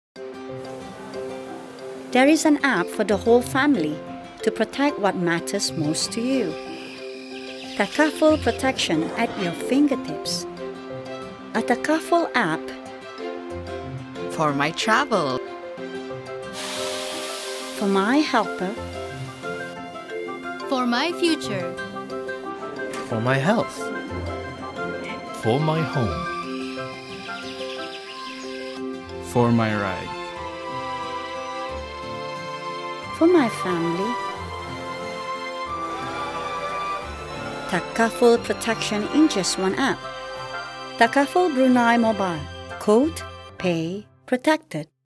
Takaful Brunei Mobile
Protecting what matters to you is important. We also know that a lot of things matter in our daily lives.

Which is why Takaful Brunei Mobile was developed to help you manage all your Takaful Certificates and give you that ease of participation, renewals, claims and more. Peace of mind at your fingertips.

Quote. Pay. Protected.